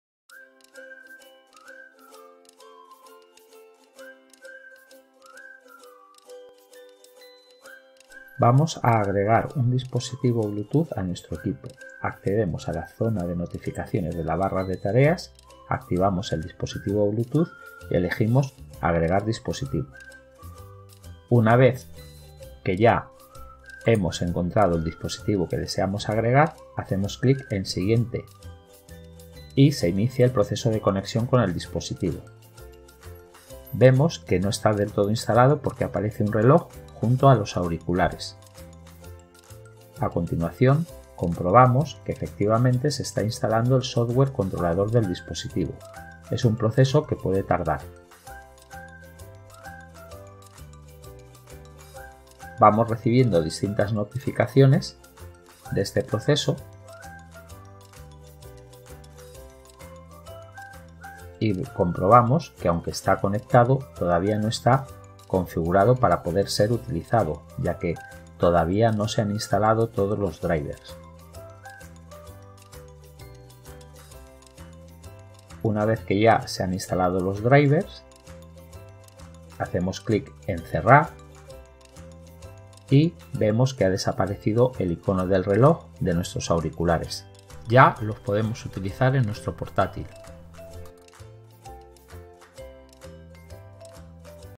Emparejar auriculares al PC mediante bluetooth
Tutorial para instalar unos auriculares bluetooth en un PC